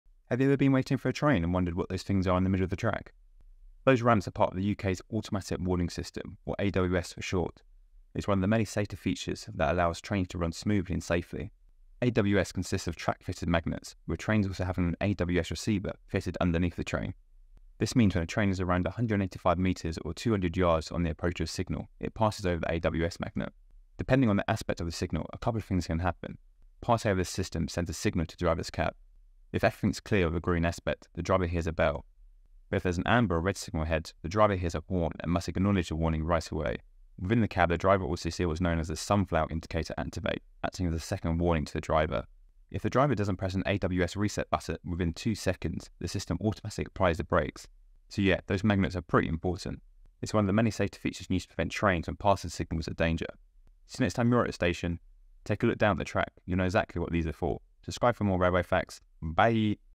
AWS what?!
Ever notice those mysterious yellow boxes on the train tracks? They’re not just random bits of metal. Welcome to the UK’s AWS – the Automatic Warning System, a hidden hero of railway safety that keeps things on track (literally)! Discover what happens when a train rolls over one of these, and how it keeps drivers sharp and passengers safe. Let’s dive into the secrets of railway tech in less than a minute! ⚠️